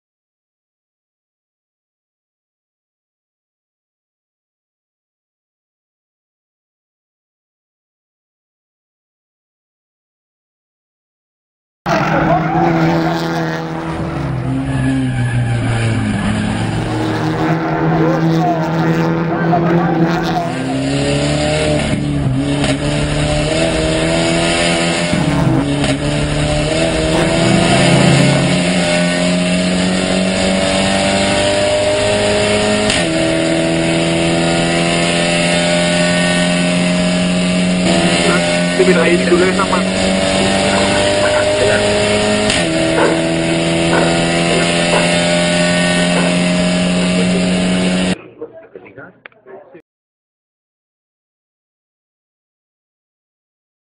ACCIDENTE DE ALEX POPOW CON RUBEN GARCIA NOVOA ACELERADOR A FONDO.wmv
fuerte accidente de Alex popow en el autodromo Moises Solana de Pachuquilla Hidalgo , fuerte ceroy manejo antideportivo de Ruben Garcia Novoa,